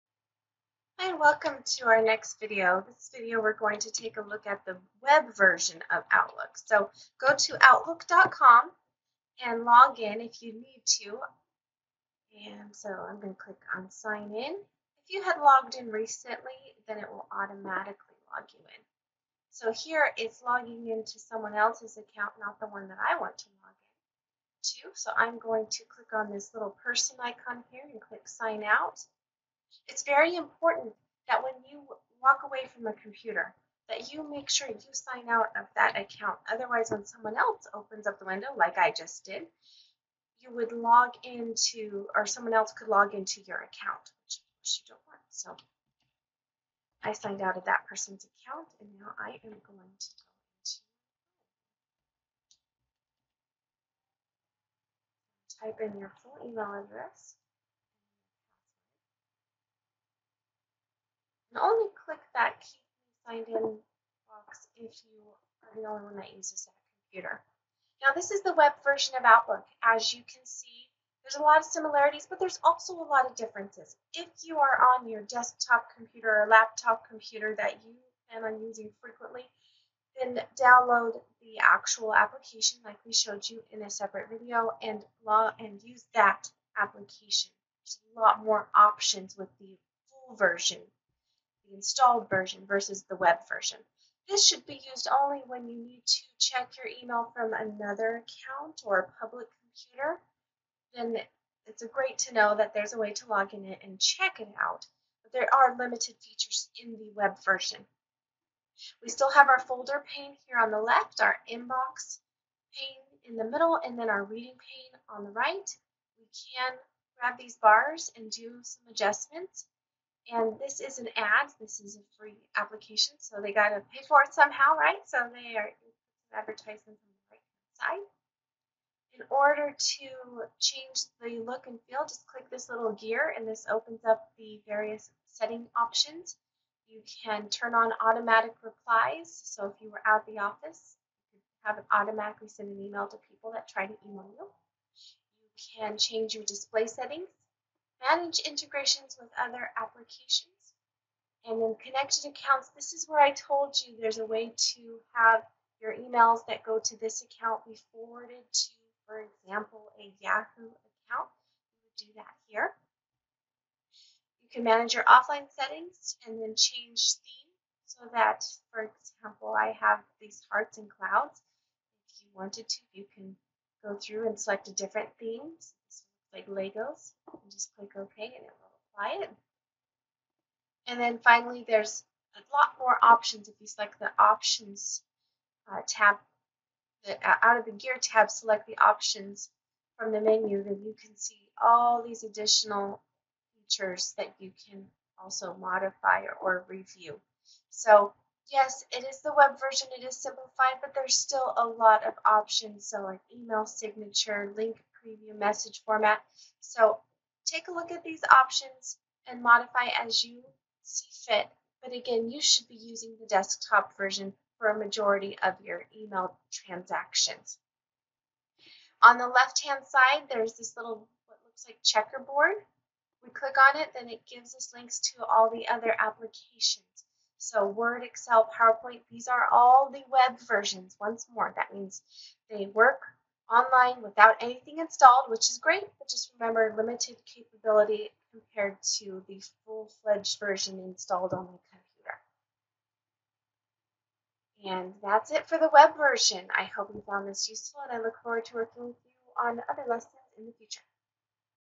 Web Version of Outlook
This reviews the web version of Outlook